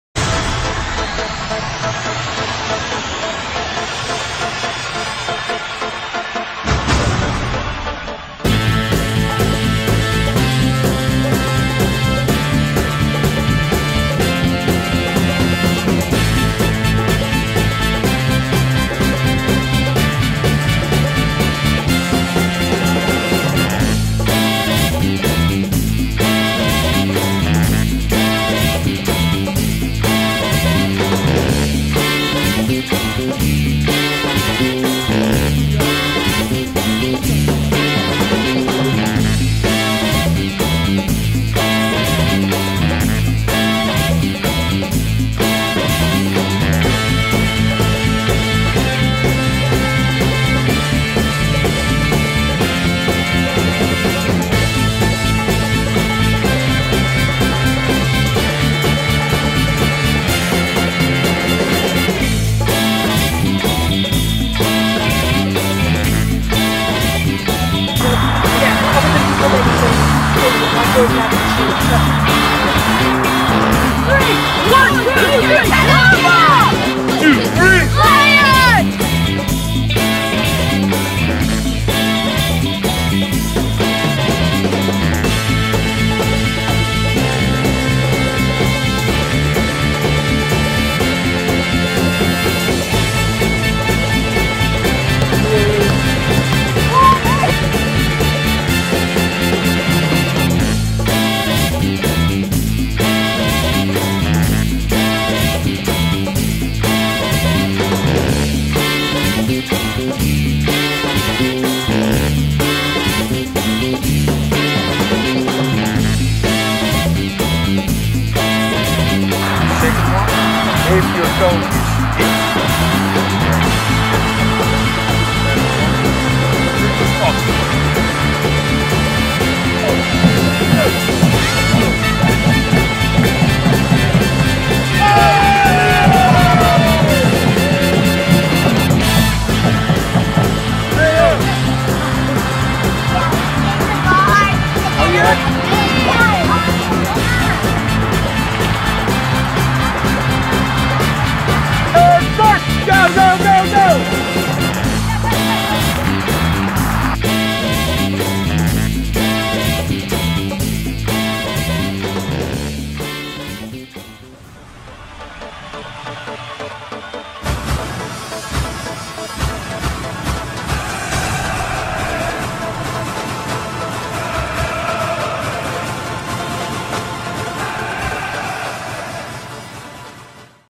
Spring Break Soccer Camp 2015
The Houston Dutch Lions FC hosted a very successful and fun Technical Soccer Camp during Spring Break at the Houston Dutch Lions FC Soccer Facility.